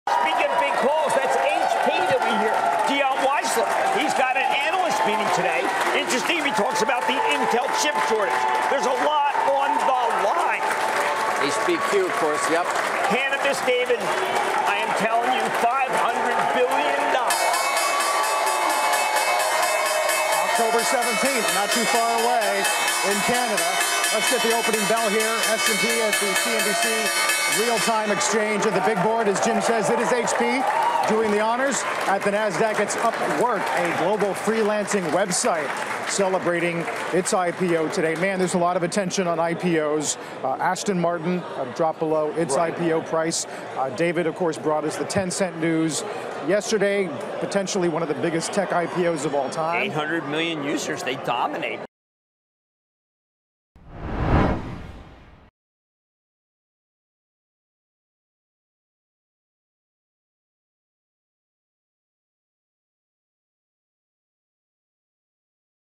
Opening Bell, October 3, 2018
Ringing today's opening bells are HP Inc. with Dion Weisler, president and CEO, at the NYSE, and Upwork with Stephane Kasriel, president and CEO, at the Nasdaq.